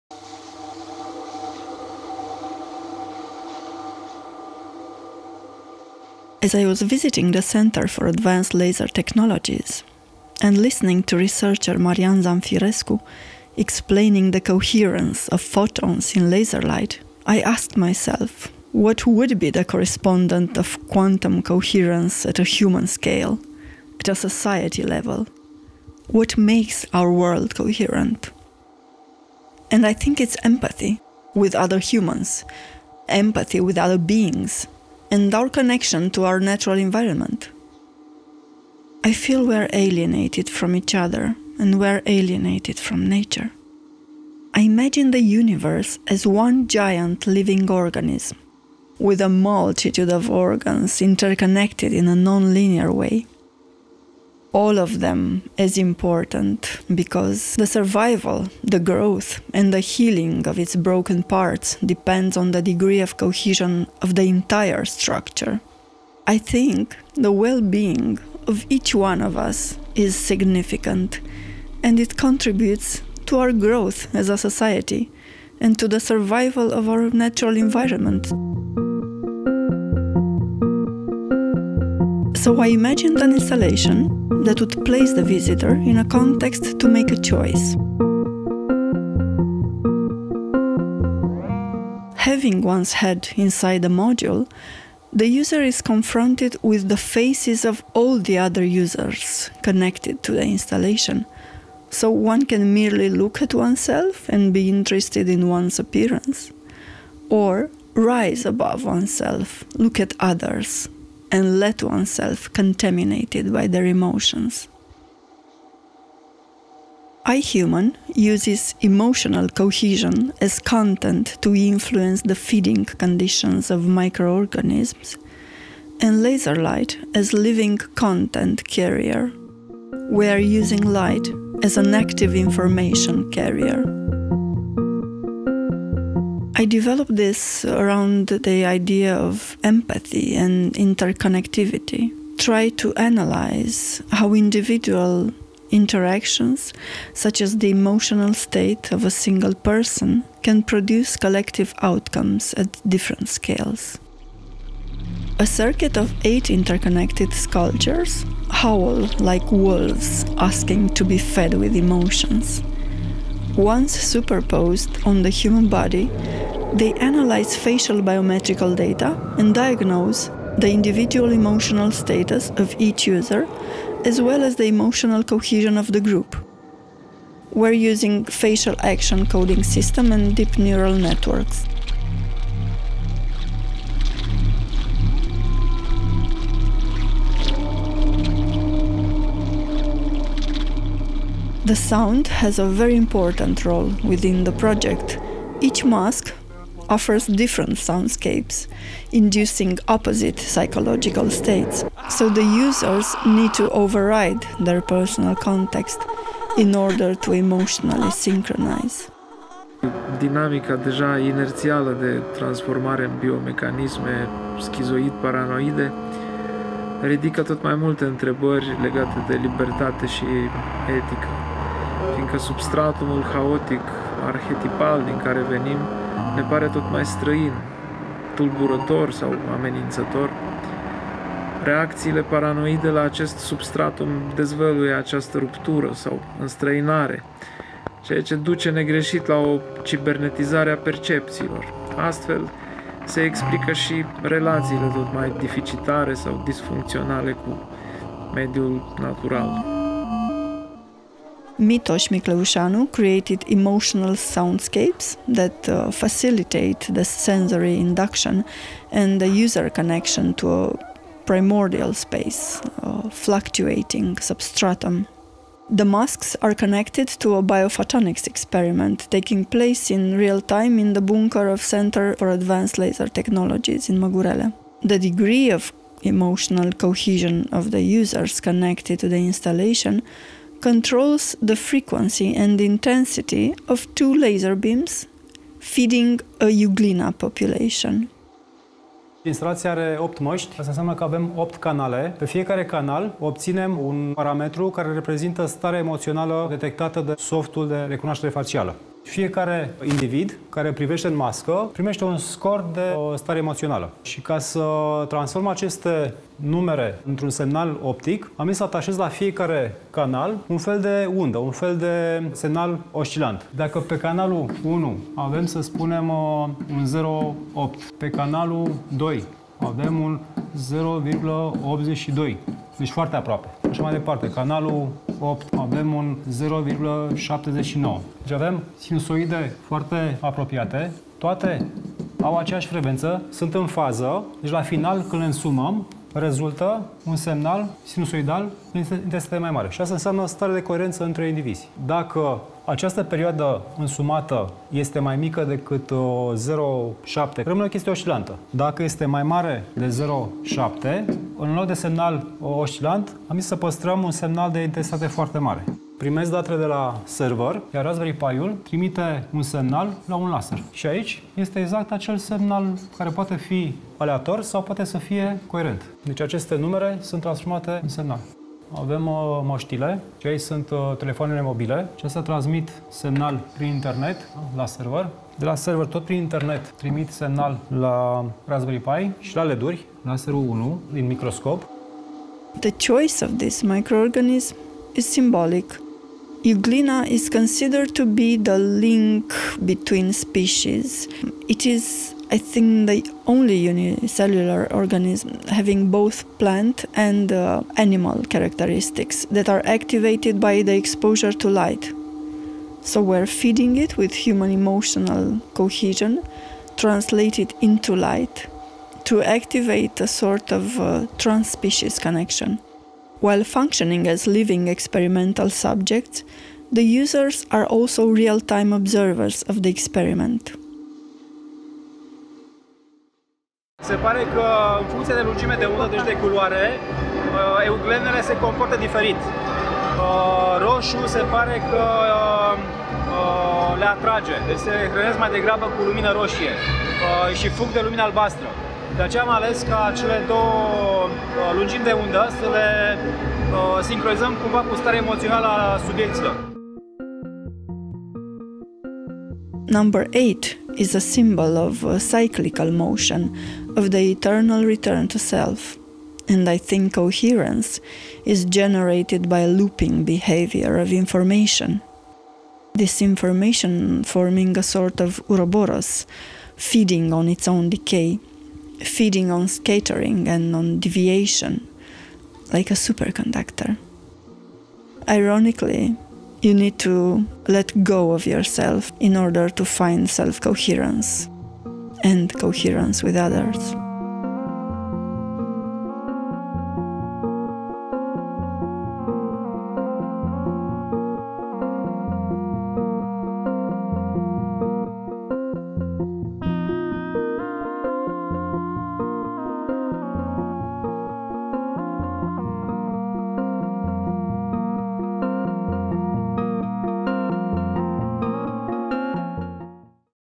I, HUMAN
I, Human, un proiect semnat de artista Saint Machine, produs de Asociaţia TRA şi CETAL şi organizat de ARCUB. După Noaptea Muzeelor, expoziţia I, Human poate fi vizitată la ARCUB, în pasajul Gabroveni şi Sala Bolţilor, în perioada 13 iunie – 7 iulie 2021, zilnic, în intervalul orar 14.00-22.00.

Cu participări la Festivalul de Artă New Media și Audio-Vizuală MADATAC (Madrid, Spania), Festivalul Ars Electronica (Linz, Austria) şi Athens Digital Arts Festival (Atena, Grecia), proiectul I, Human, construit în jurul conceptului de interconectivitate, este prezentat la ARCUB în contextul post pandemic, în care ARCUB caută să reactiveze spaţiul de întâlnire dintre bucureşteni şi artiştii oraşului afectaţi de restricţiile impuse din ultimii ani.

Gândit ca o instalaţie media interactivă, I, Human testează, prin artă și tehnologie, noi modele de comportament colectiv. Proiectul semnat de Saint Machine propune o rețea de sculpturi luminoase ce funcționează drept colectori de emoții umane, dar și drept agenți generatori ai acestora, vizitatorii fiind influențați emoțional de peisajul sonor diferit din fiecare modul.

„I, Human analizează impactul pe care o acţiune individuală, locală, ce poate părea neînsemnată, cum este starea psihică a unui singur individ, poate afecta structuri colective. În mod simbolic, am folosit empatia utilizatorilor instalației drept conținut pentru a influența condițiile de hrănire ale unei populații de microorganisme și lumina laser drept mediu purtător de conținut, sau altfel spus, informație. Vizitatorii expoziției sunt implicați ca subiecți umani ai experimentului, generatori de conținut, dar și observatori în timp real ai acestuia.” declară artista Marilena Oprescu Singer (aka Saint Machine).

Cele 8 module interconectate ale instalației detectează astfel emoțiile privitorilor și transformă gradul lor de empatie în pulsuri de lumină laser ce hrănesc o populație de microorganisme în cadrul unui experiment de biofotonică desfășurat în timp real.

La graniţa dintre instalaţie multimedia, performance și studiu al coeziunii umane mediate de tehnologie, Saint Machine a creat proiectul transdisciplinar I, Human în colaborare cu cercetătorul Marian Zamfirescu și echipa CETAL – Centrul de Tehnologii Avansate cu Laseri din cadrul Institutului Național pentru Fizica Laserilor, Plasmei și Radiației. Din echipa proiectului I, Human fac parte compozitorul Mitoș Micleușanu, programatorii Alexandru-Florin Vartic, Sorin Olexiuc, Răzvan Levarda și inginerul electronist Radu Mititelu.

I, Human a mai fost prezentat cu sprijinul Institutului Cultural Român la nivel internațional la ARS ELECTRONICA 2019 în cadrul expoziției Limited Humanity – Platforma Europeană pentru Umanism Digital, la Athens Digital Arts Festival, iar pe plan local la festivalul de artă new media RADAR – Romanian Artists Developing Alternative Realities și Insatiable Machines – Halucinarium.

Partea de cercetare a instalației I, Human a demarat în august 2018 în cadrul Fusion – un program de rezidențe artistice în institutele de cercetare de la Măgurele, organizat de Fundația9. În luna februarie 2019 proiectul a fost expus în etapa de “work in progress” în cadrul expozitiei Fusion la Rezidența BRD-Scena9 cu sprijinul EXPO ARTE, apoi instalația a participat la Festivalul de Artă New Media și Audio-Vizuală MADATAC, la Medialab-Prado, Madrid.

De asemenea, procesul de lucru și partea de cercetare artistică a proiectului I, Human a fost prezentată în cadrul Encuentro AVLab – Albert Merino y Saint Machine la Medialab-Prado.

Partener de cooperare: CETAL – Centrul de Tehnologii Avansate cu Laseri din cadrul INFLPR –Institutul Național de Cercetare-Dezvoltare pentru Fizica Laserilor, Plasmei si Radiației

Credit video: Saint Machine